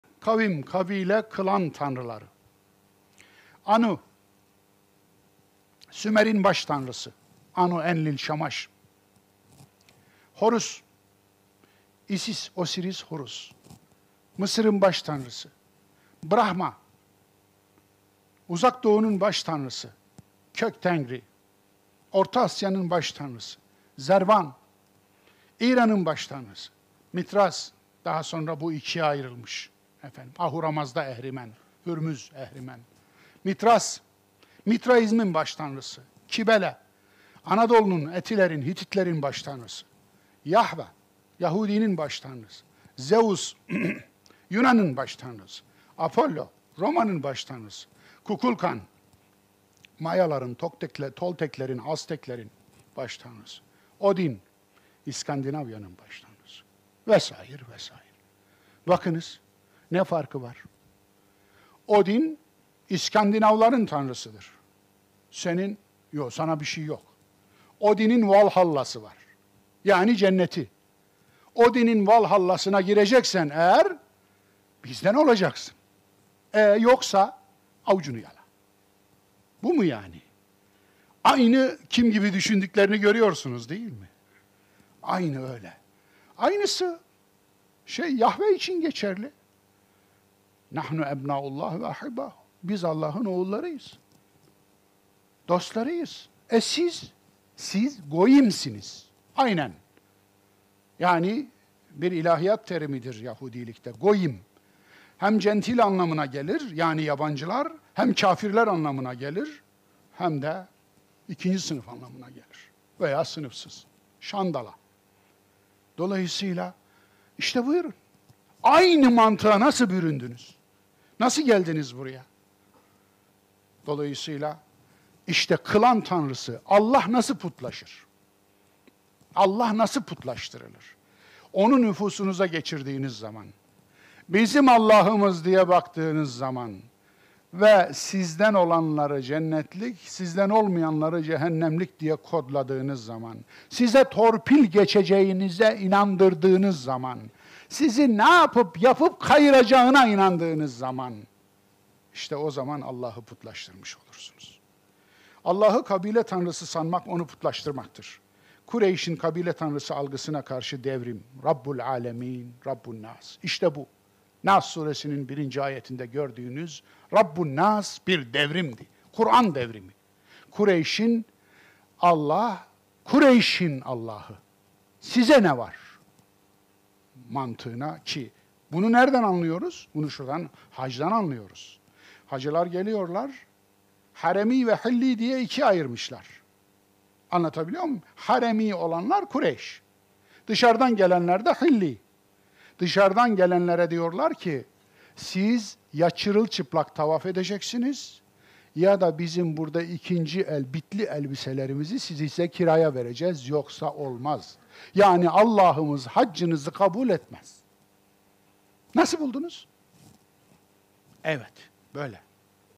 Kavim, kabile, klan tanrıları
27 Kasım 2022 tarihli [Kur’an’ın Hayat Yürüyüşü 46. Ders | Nas Suresi: Bir “Kendini Tanı” Dersi] başlıklı programdan alıntılanmıştır.